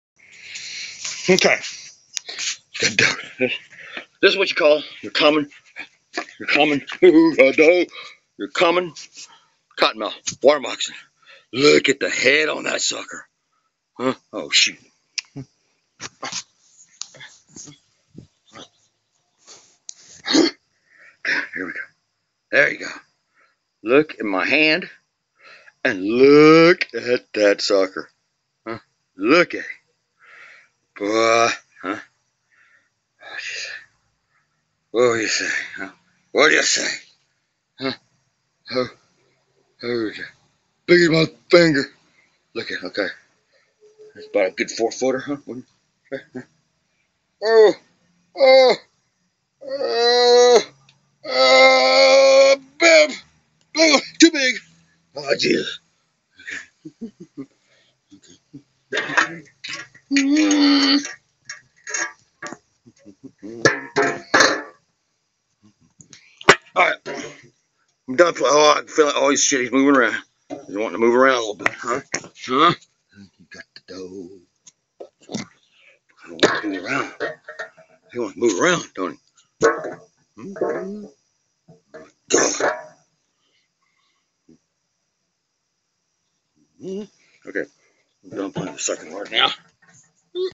Biggest Cotton Mouth Ever Seen
5 to 6 foot cotton month almost bit me while frog gigging in East TX.